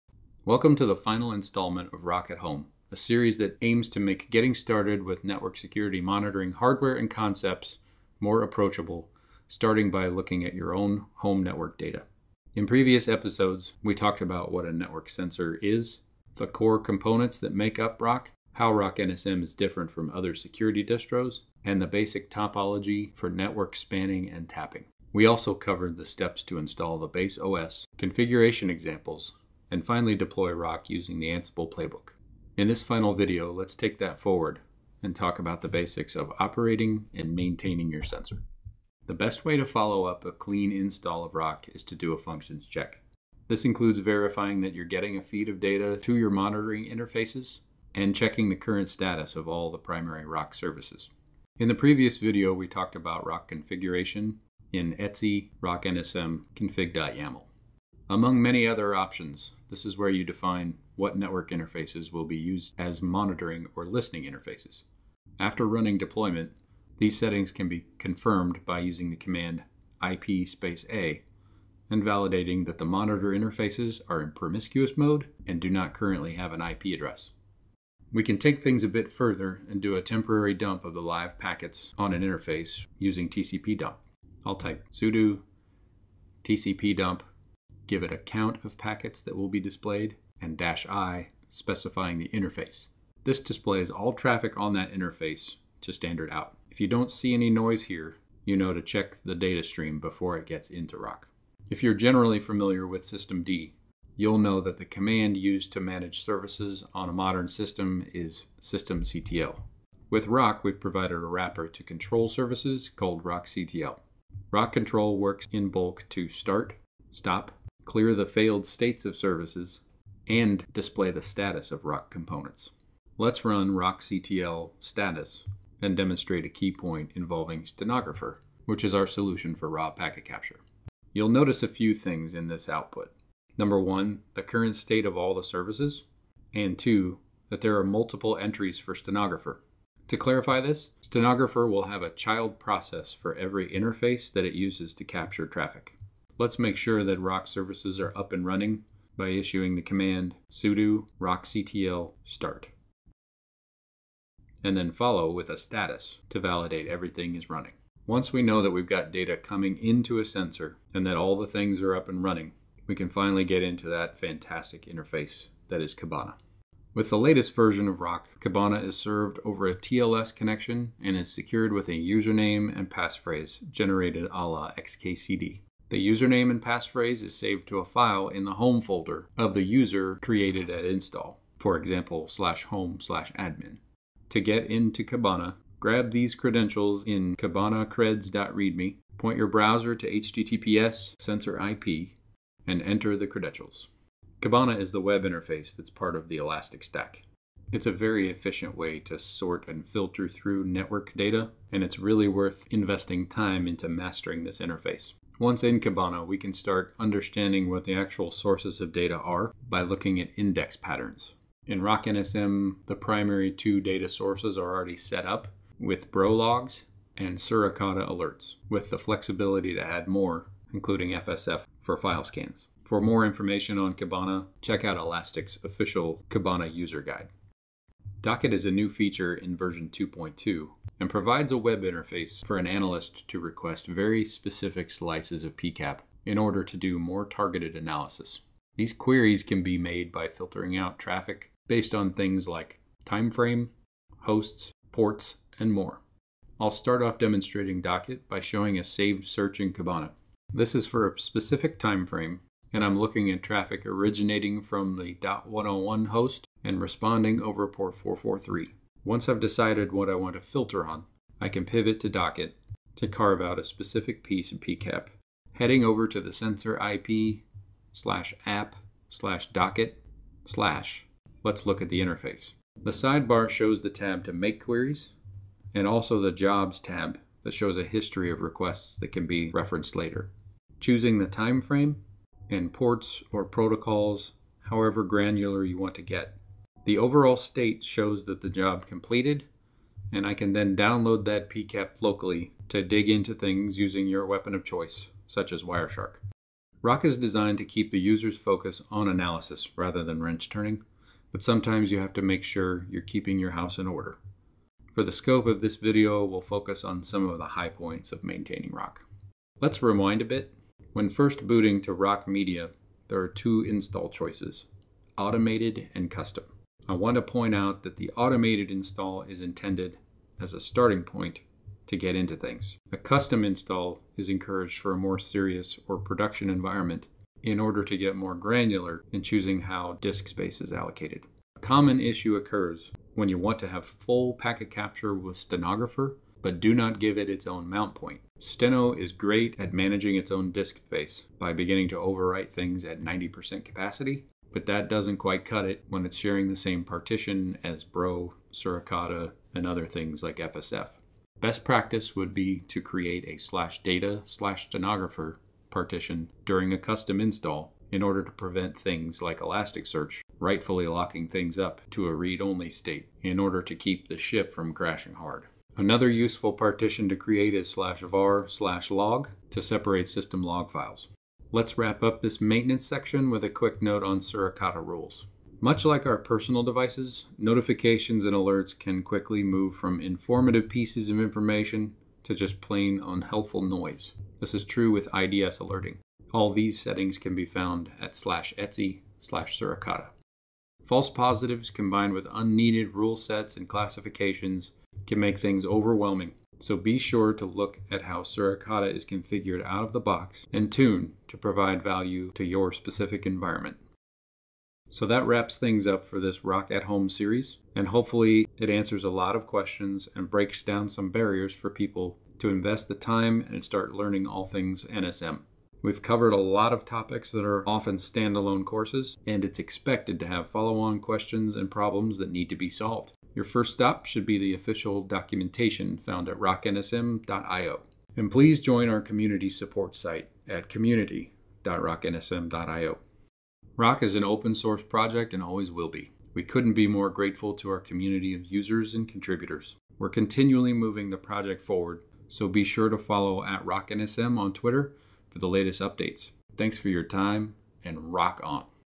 ROCK@home part 3 - Operate and Maintain
This episode covers basic operation, maintenance, and general troubleshooting of RockNSM.  This is the last a 3 part series on how to use ROCK to collect network traffic data on your own network.